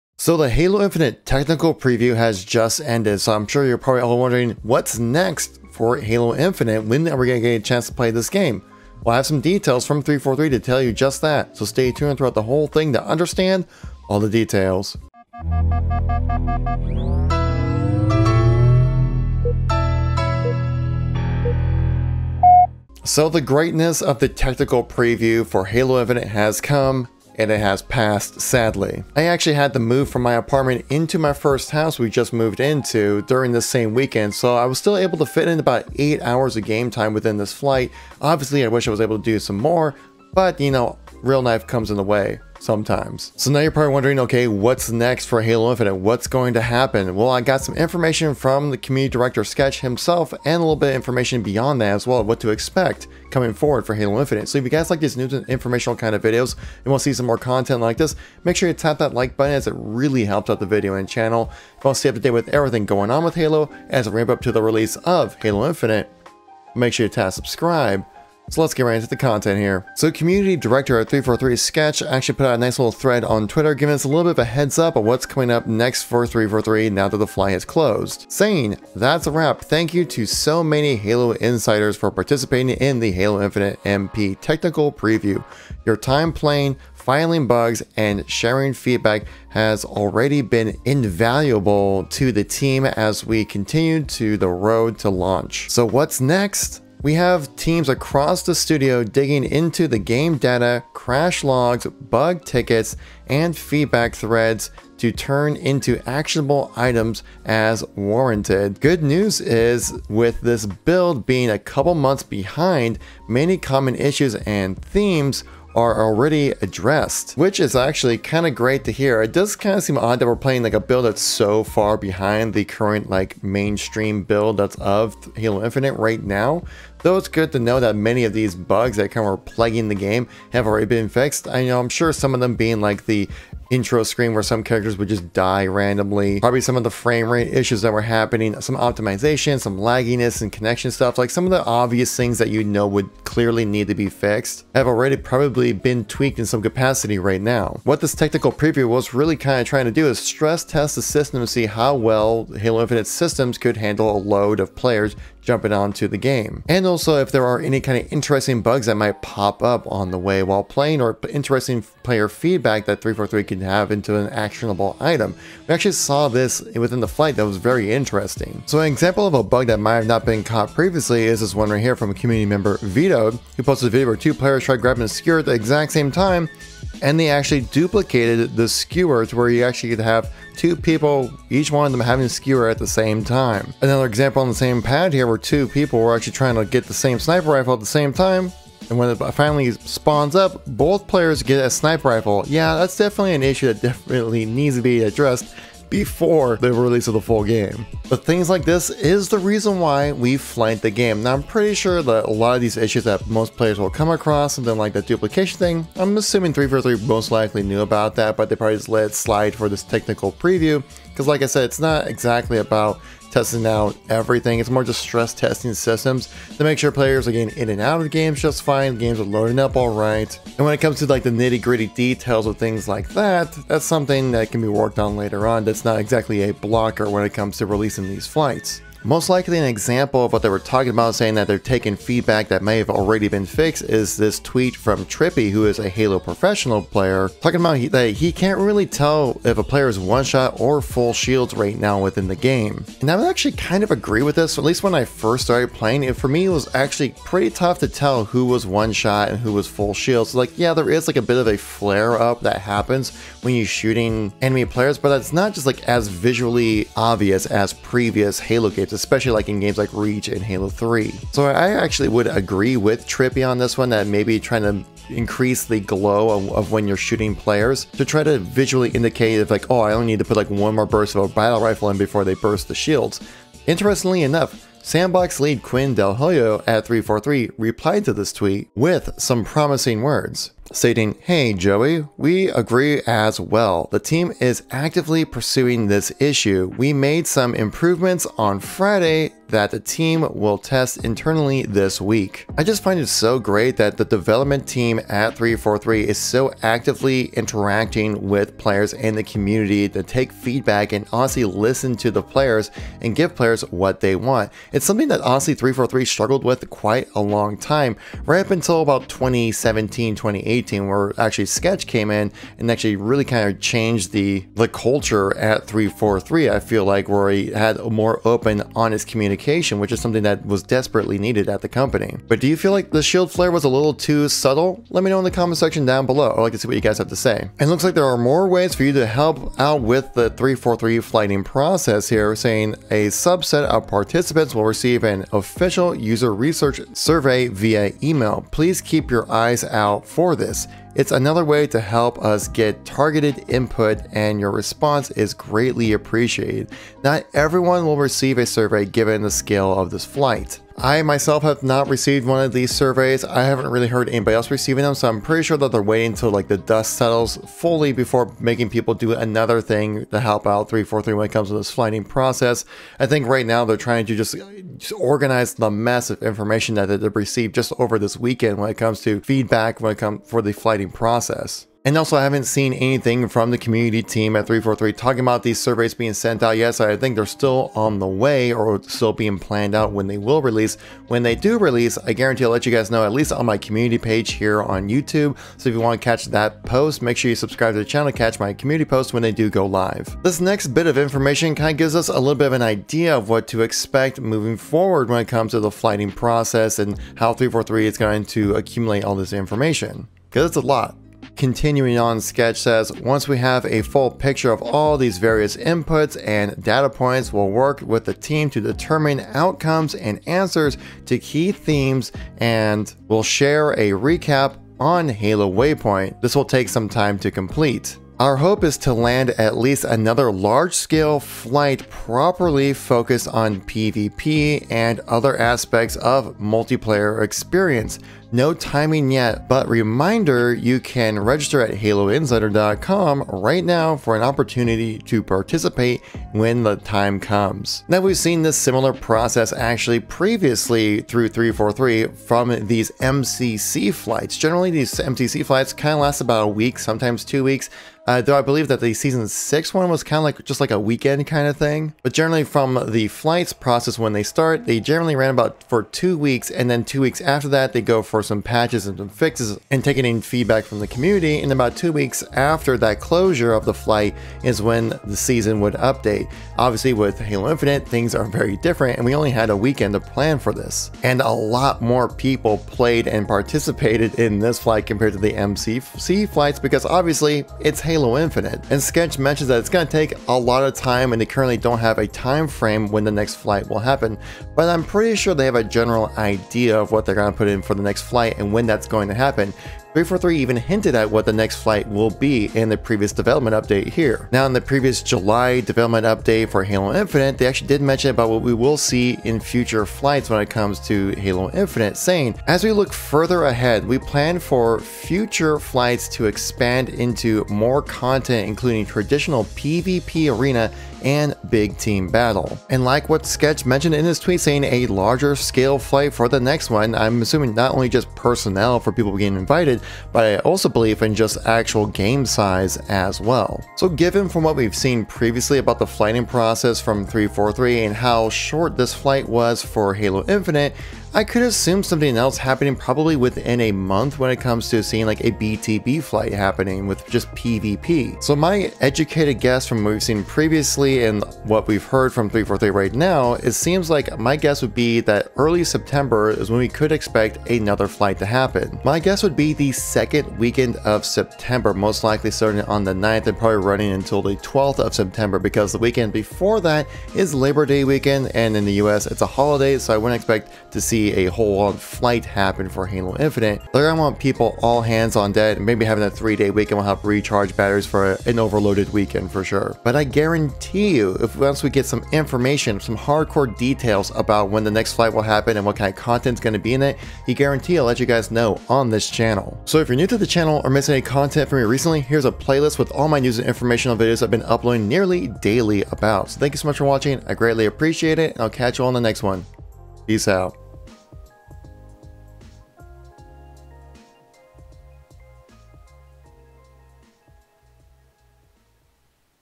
What's Next For Halo Infinite After Technical Preview? PVP and BTB Flight! Halo Infinite News
Timestamps
Intro - 0:00
What's Next for Halo Infinite - 1:09
Next Flight When Info - 6:29

Sources
What Next by Ske7ch

What's Next For Halo Infinite After Technical Preview? PVP and BTB Flight Next! Halo Infinite News